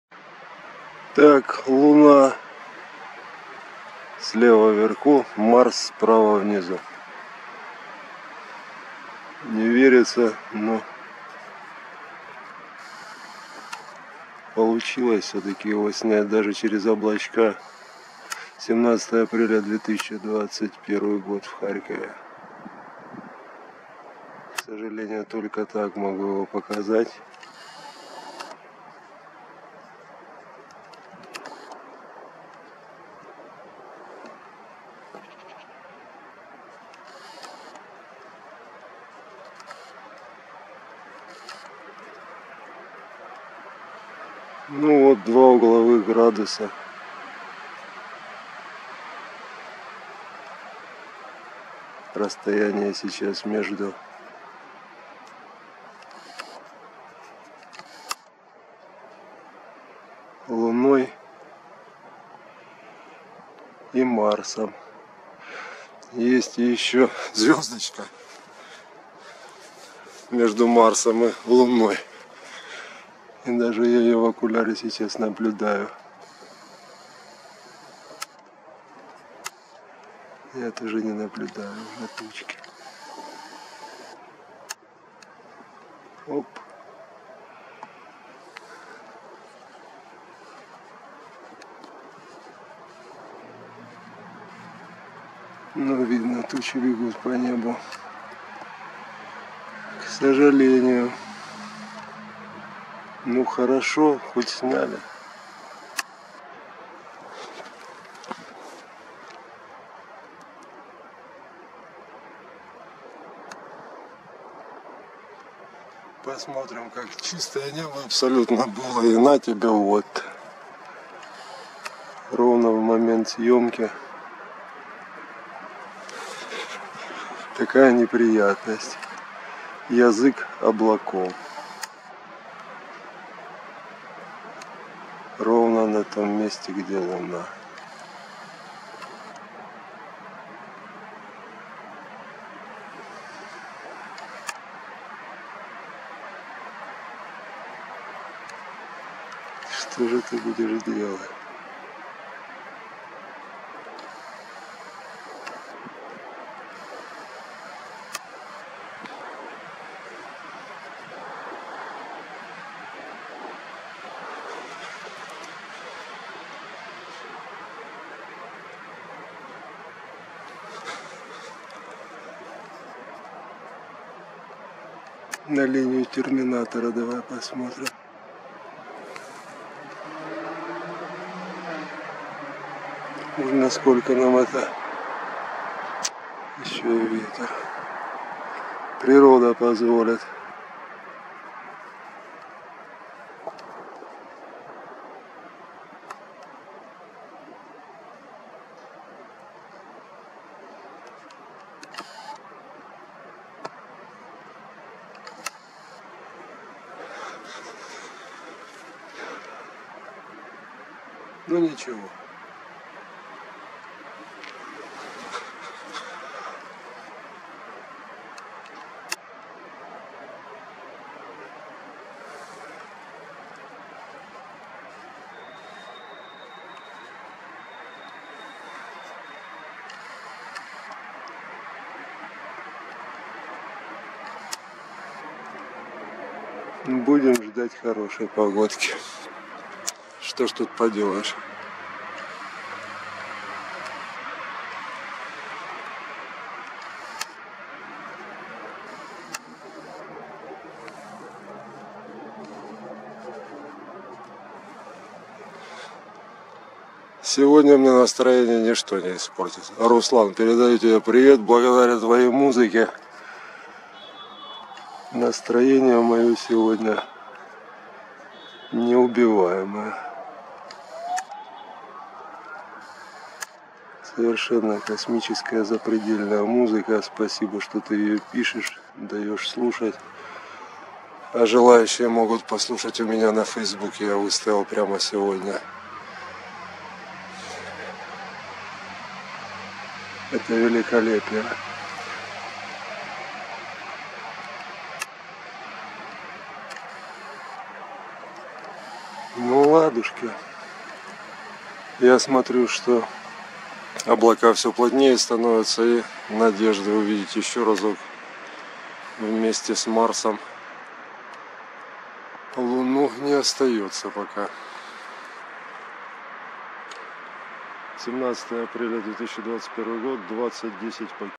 Луна и Марс 17 апреля 2021
Молодой Месяц и Марс в двух угловых градусах от него 17 апреля 2021.